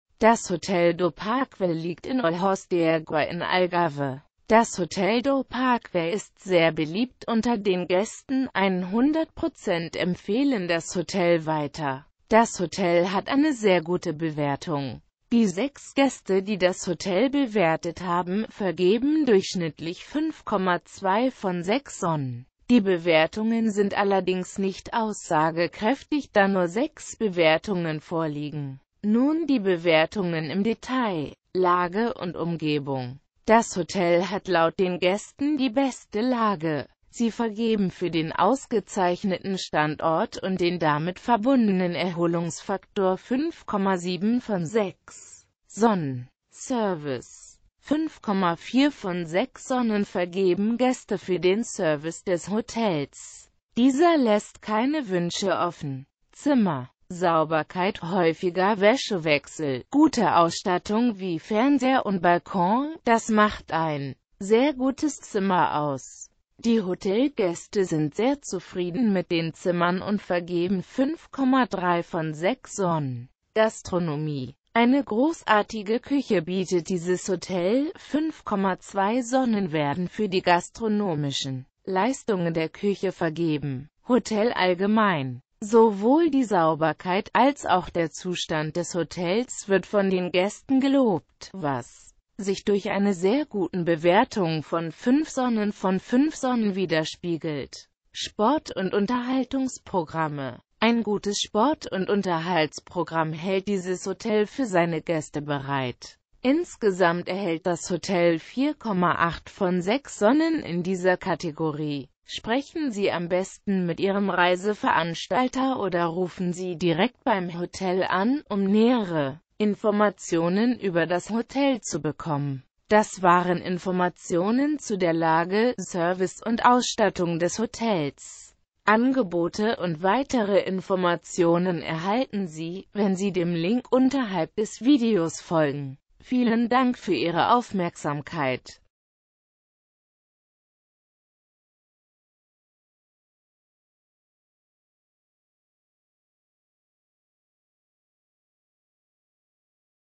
Hotel do Parque in Olhos de Agua (Algarve - Portugal) Bewertung und Erfahrungen
Das Hotel do Parque liegt in der Stadt Olhos de Agua welche zu Portugal (Europa) gehoert.

 Das Hotel do Parque ist sehr beliebt unter den Gaesten, 100% empfehlen das Hotel weiter. Zudem erhaelt das Hotel 5.2 von 6 Sonnen. Die Hotelbewertungen sind allerdings nicht aussagekraeftig, da nur 6 Bewertungen vorliegen. Sprechen Sie am besten mit Ihrem Reiseveranstalter oder rufen Sie direkt beim Hotel an, um mehr Informationen ueber das Hotel zu bekommen.
Lage und Umgebung:
 Das Hotel hat laut den Gaesten die beste Lage. Sie vergeben fuer den ausgezeichneten Standort und den damit verbundenen Erholungsfaktor 5,7 von 6 Sonnen.
Service:
 5,4 von 6 Sonnen vergeben Gaeste fuer den Service des Hotels. Dieser laesst keine Wuensche offen.
Zimmer:
Gastronomie:
Hotel allgemein:
 Sowohl die Sauberkeit, als auch der Zustand des Hotels wird von den Gaesten gelobt, was sich durch eine sehr guten Bewertung von 5 Sonnen von 5 Sonnen wiederspiegelt.

Informationen zur Verfuegbarkeit von Transfer zum Hotel, WLAN in den Zimmern des Hotel do Parque, sowie Details zum Animations-Progamm, Entfernung zum Strand oder zu Diskotheken erhalten Sie bei Ihren Reiseveranstalter oder auf der Webseite des Hotels. Neben den hier verwendeten Bewertungen, lohnt sich ein Blick auf die ggf. vorhandene Hotel do Parque Facebook-Seite.

Quellenhinweis:
Die verwendeten Informationen stammen von HolidayCheck.de. Sie wurden am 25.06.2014 abgerufen. In diesem Video wurden Bilder aus Olhos de Agua oder allgemeine Bilder aus Portugal verwendet. Die gezeigten Orte und Landschaften dienen als Beispiel und muessen nicht unbedingt in der Naehe des Hotels liegen.